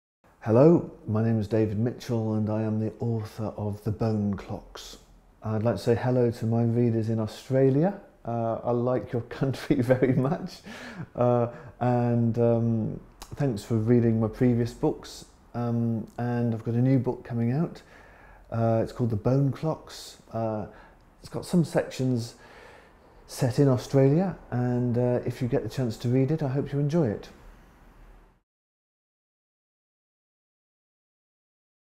A message to David Mitchell's Australian fans
THE BONE CLOCKS will be in store September 2.

LONGLISTED FOR THE MAN BOOKER PRIZE 2014. This kaleidoscopic novel crackles with the invention and wit that have made David Mitchell one of the most celebrated writers of his generation. Here is fiction at its most spellbinding and memorable best.